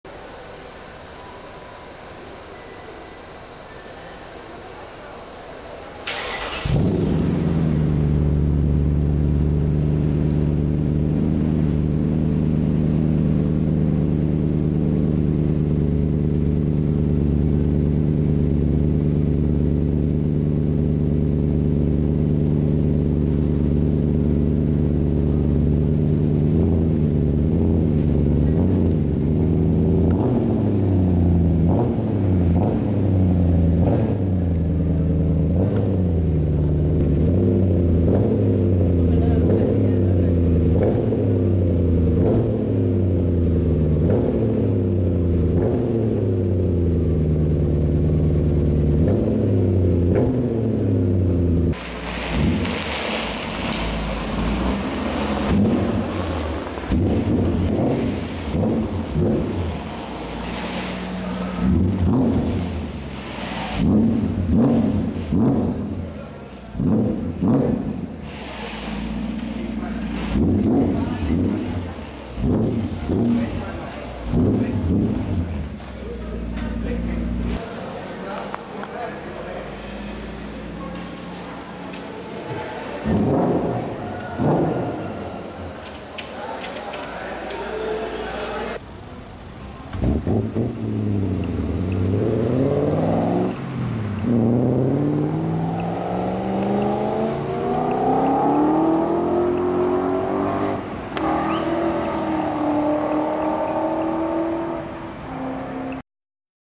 BMW M5 V8 (E39)
VITAL SPEC SHEET

BMW M5

• V8, 4941cc
• 0-100km/h in 4.9 seconds
• 0-200km/h in 16.1 seconds
• 323Kw
• 534Nm
• Rob Green Motorsport Stage 1 Conversion
• BMC Intake
• Custom RGM stainless steel performance exhaust
• Cat-less header, custom branches
• UniQ chip
• Governor removed, 300km/h+
• 1:27:0 around the Top Gear Track. Faster than a KTM X-Bow, Aston Martin DB9 & M3 CSL.
• 8:28 around Nurburgring. Faster than GT-R R33, CLK 55 AMG and Porsche 993 Carrera.